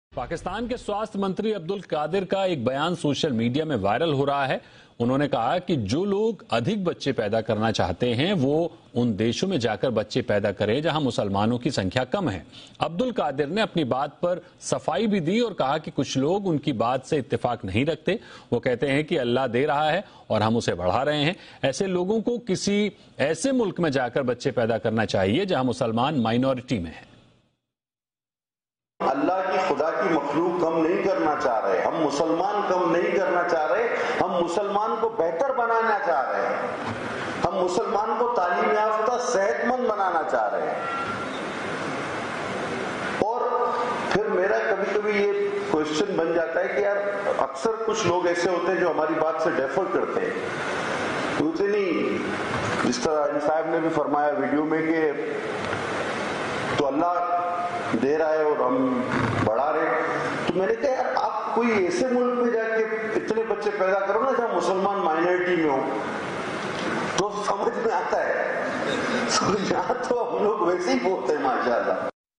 An ‘out of Pakistan’ solution for population control—produce kids in non-Muslim nations
पाक के स्वास्थ्य मंत्री का बेतुका बयान - दूसरे देश जाकर बच्चा पैदा करें पाकिस्तानी